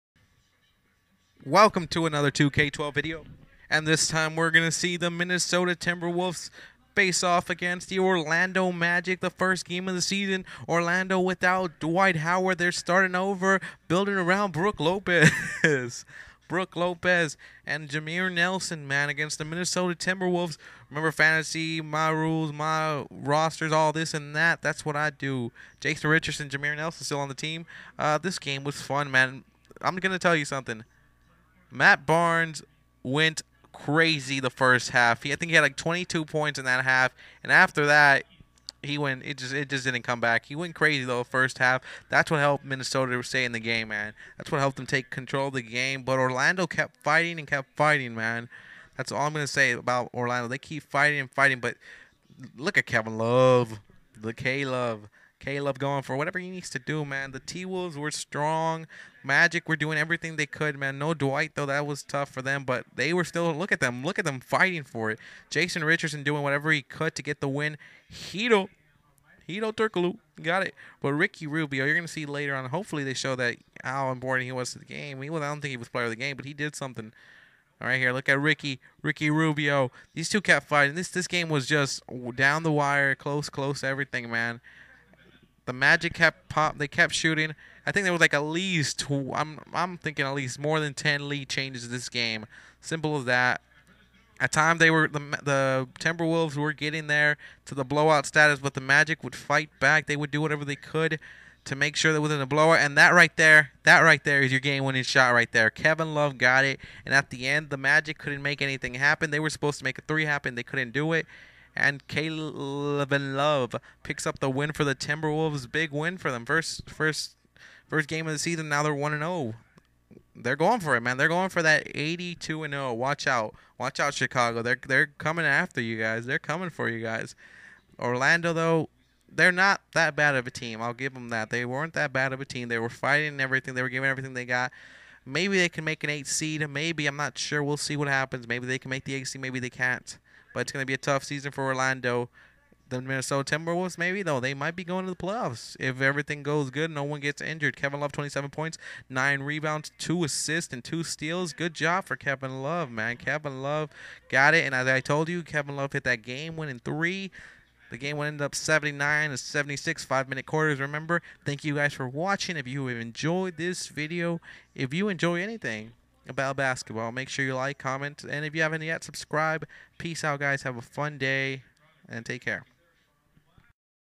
Orlando Magic .vs. Minnesota T-Wolves- Day 2
Game One post Dwight Howard Era for the Magic.